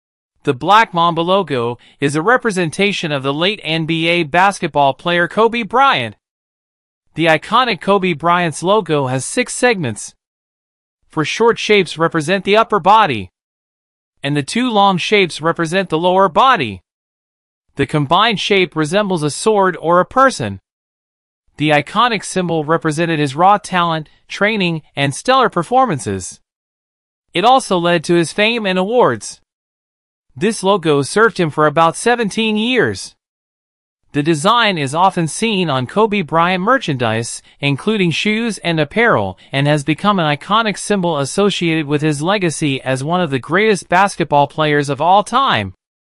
The Black Mamba logo
The iconic Kobe Bryant’s logo has six segments. Four short shapes represent the upper body. And the two long shapes represent the lower body.  The combined shape resembles a sword or a person.